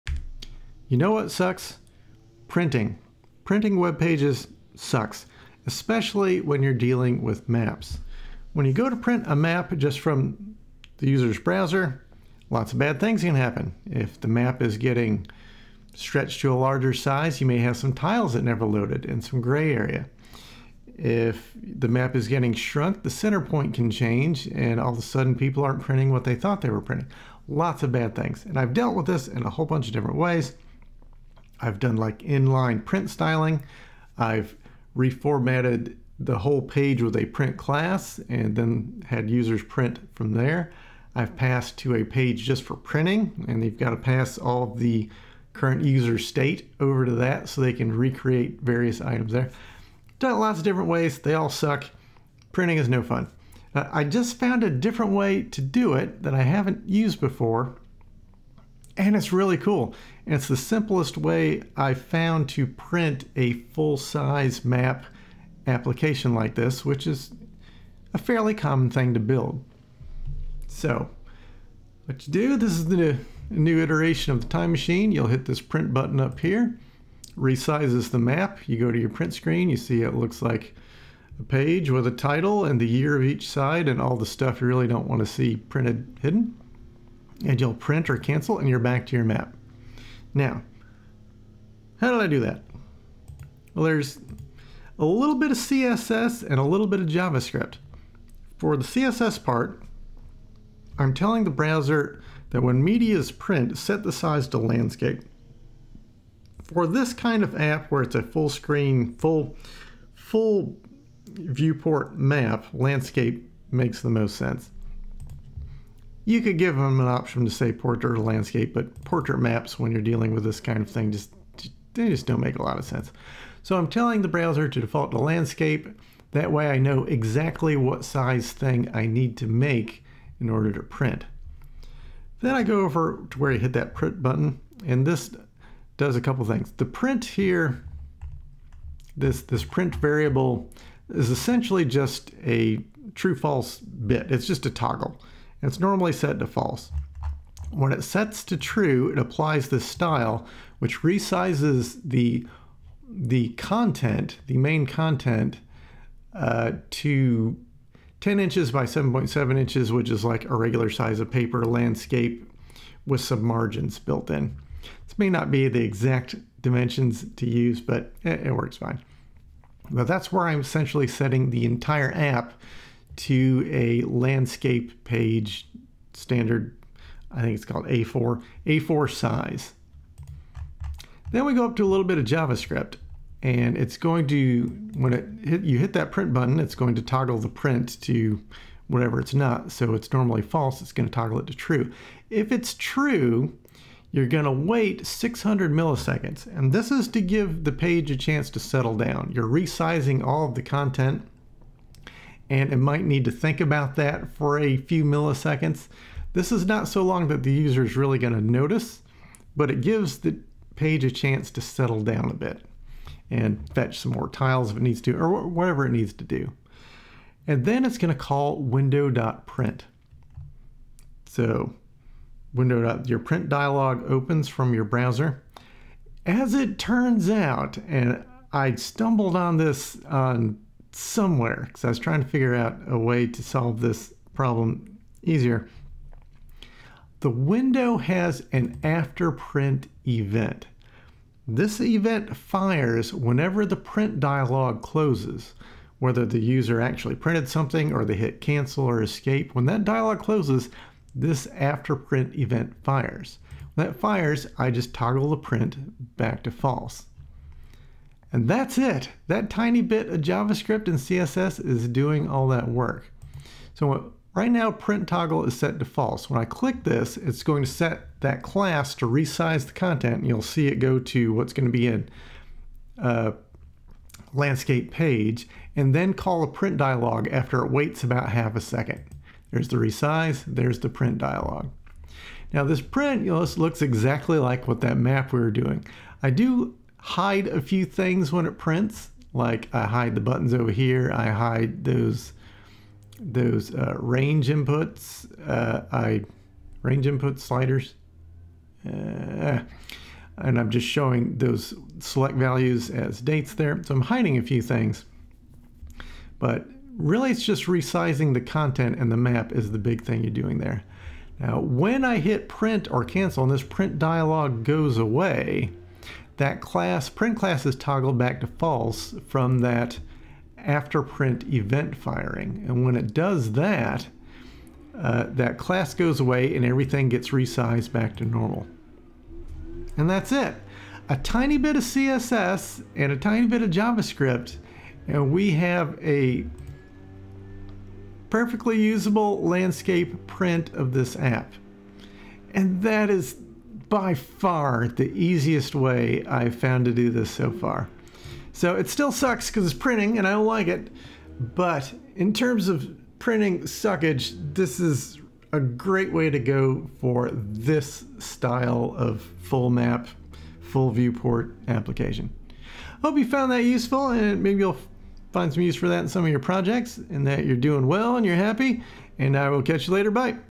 Easy printing with the afterprint event
How did I never hear about the afterprint event? It along with some simple CSS makes printing full-viewport mapping apps a lot easier.